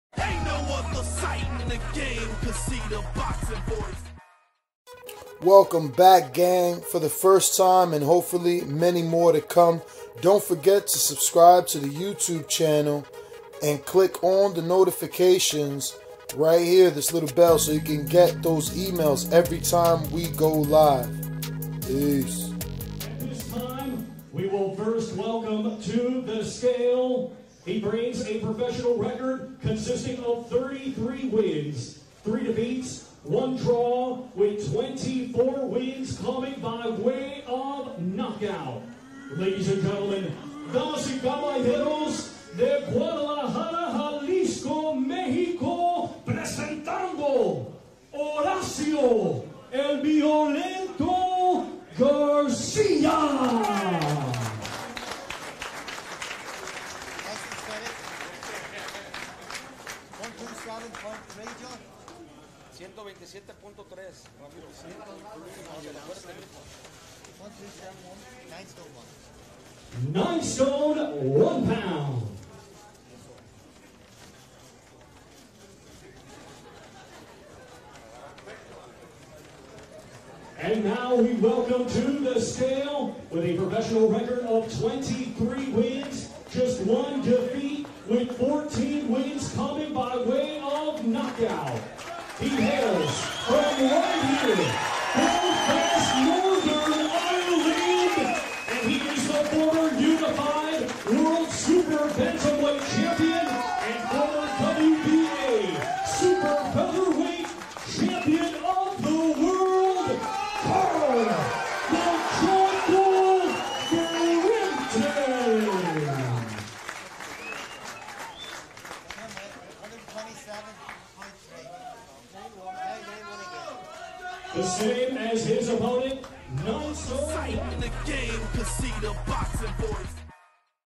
Carl Frampton vs Horacio Garcia: The Weigh-In From Belfast Northern Ireland
Carl Frampton vs Horacio Garcia: The Weigh Inn From Belfast Northern Ireland

Courtesy to BT SPORT for Video Footage